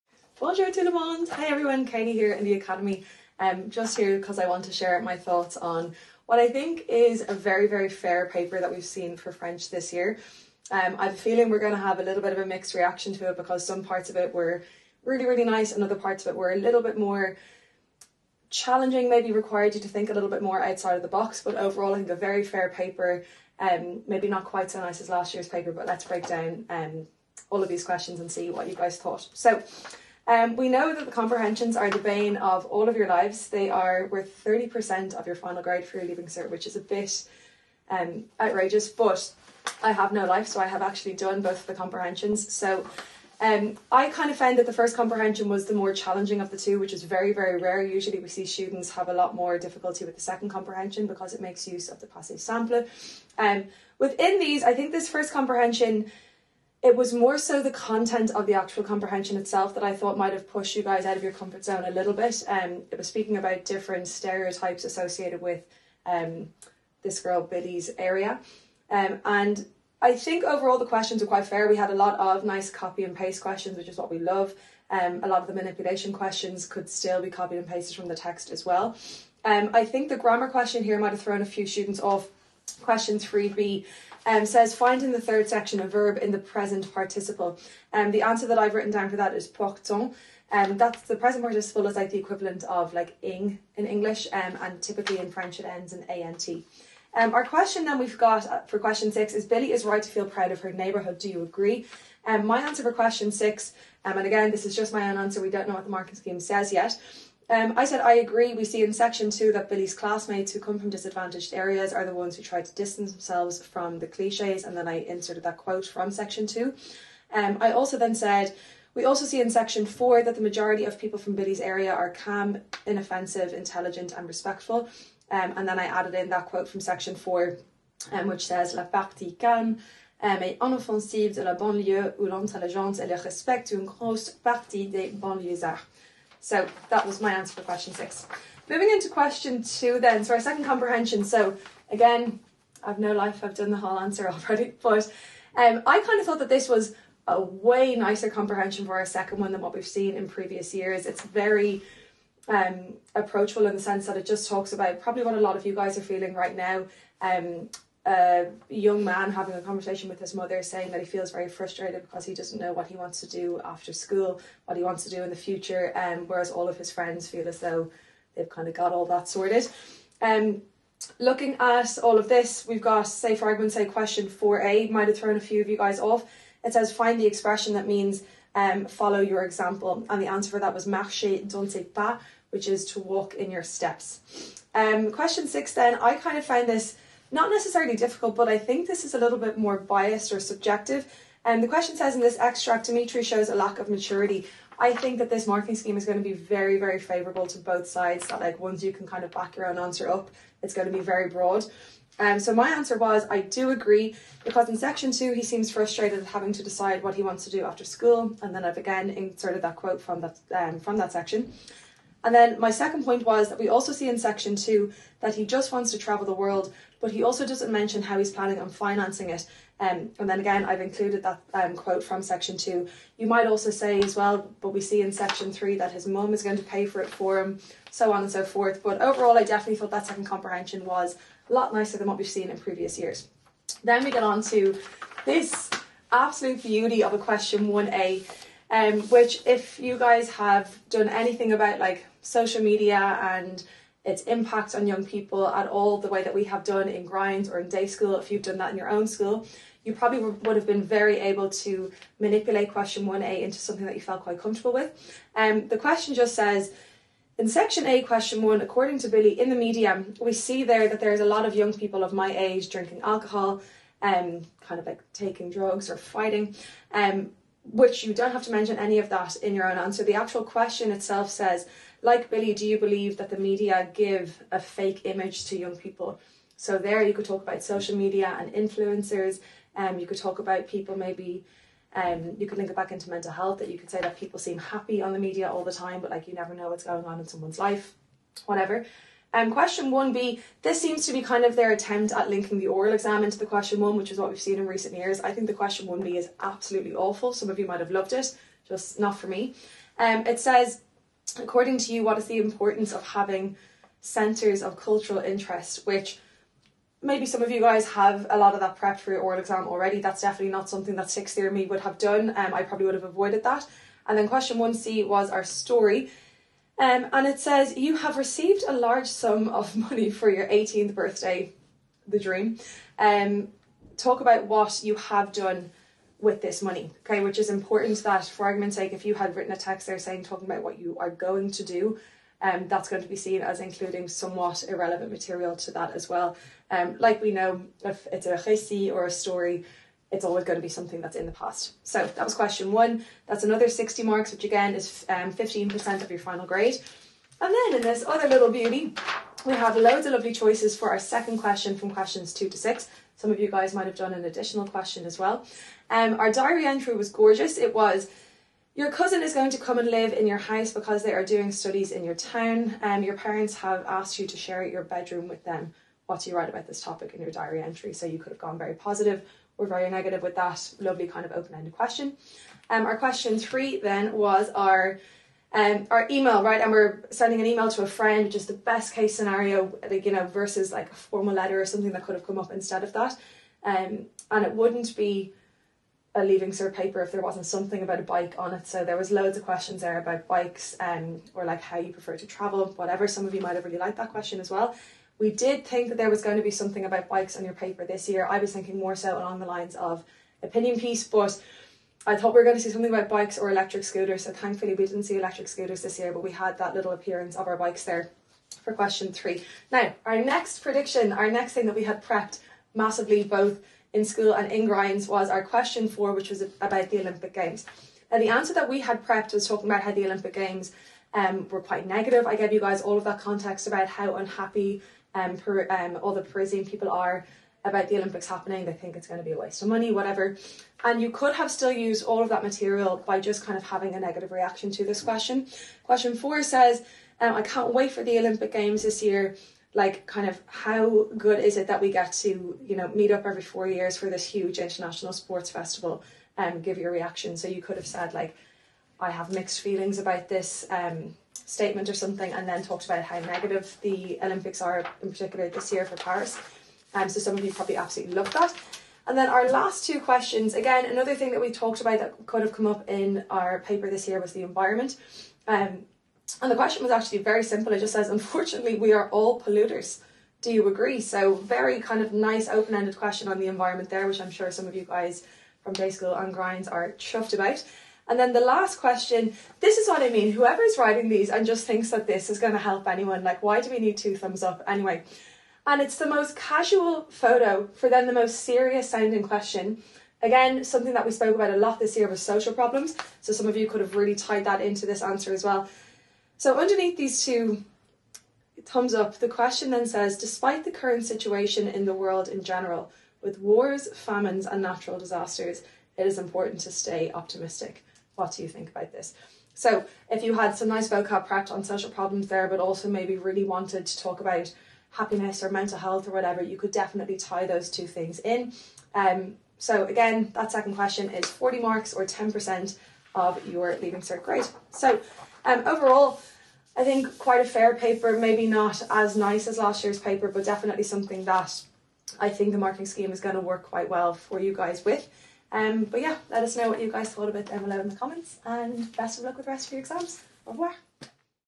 French Leaving art (H) 2024- Reaction Video
French teacher Katie gives us her reaction to the Leaving Cert French exam paper! Watch to see Katie give her opinion and breakdown of this years French exam! 🇫🇷🥖
“The thing we have prepped massively both in school and in grinds was our question 4 about the Olympic Games”